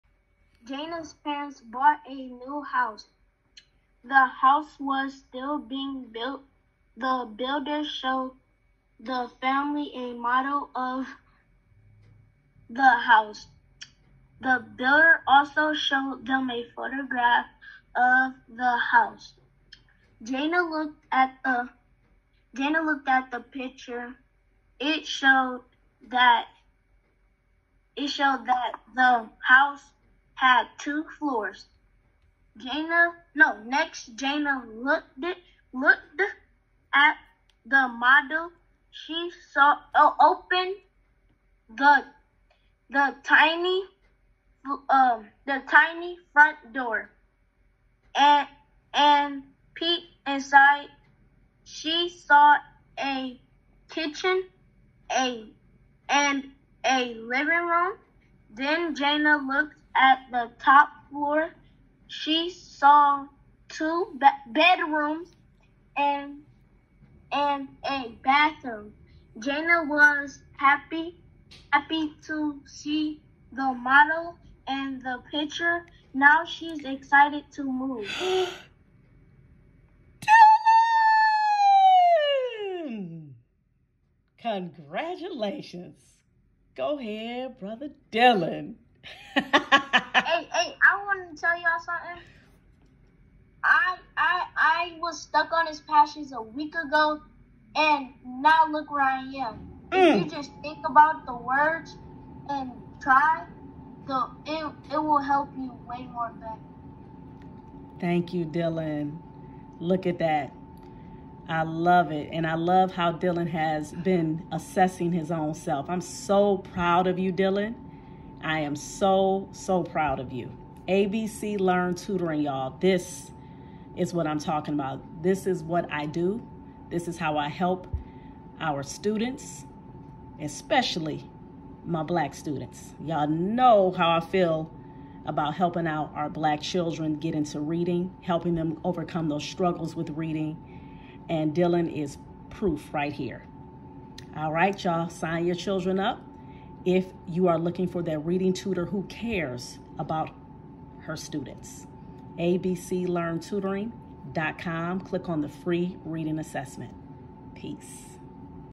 Dylan was stuck on this passage a week ago now look at him!
I’m so proud of my student Dylan. He has come along way. He is reading with more confidence, he’s taking his time, and reading more smoothly. These are just a few of the improvements that he’s made while under my tutelage. Abclearntutoring.com is the place for your pre-K-4th grade child who is struggling with reading.